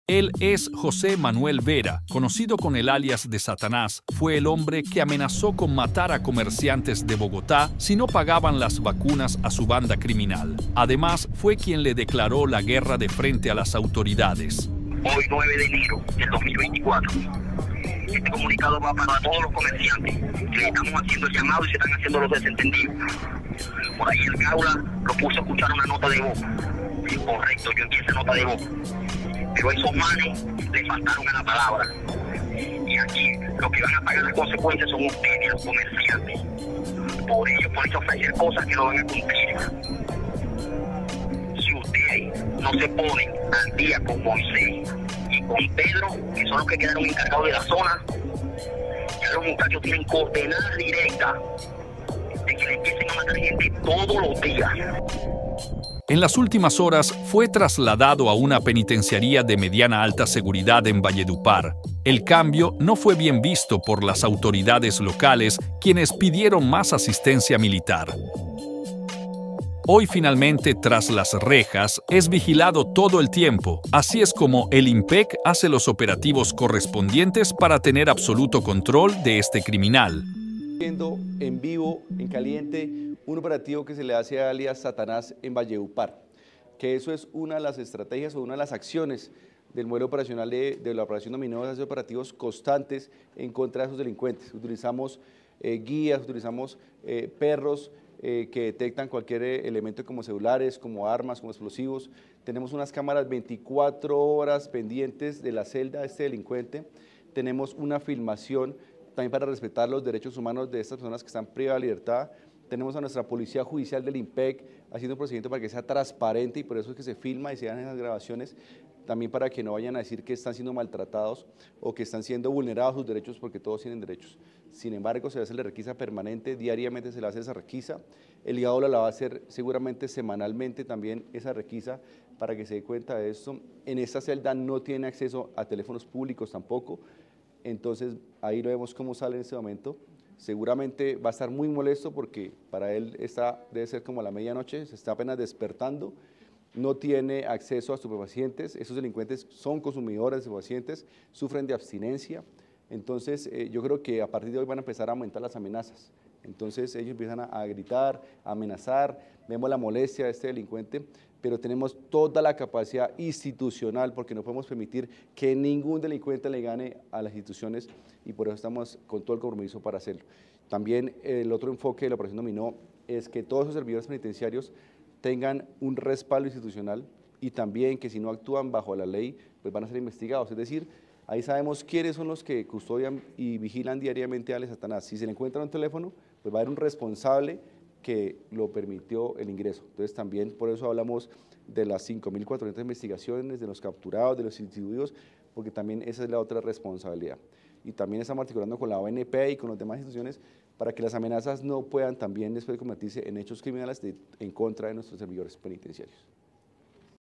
SATANÁS vive un mal momento en la cárcel.
es uno de los hombres que tiene aterrorizandos a los comerciantes en Bogotá, delinquía aún desde la cárcel, por ello fue enviado a Valledupar a una cárcel de máxima seguridad.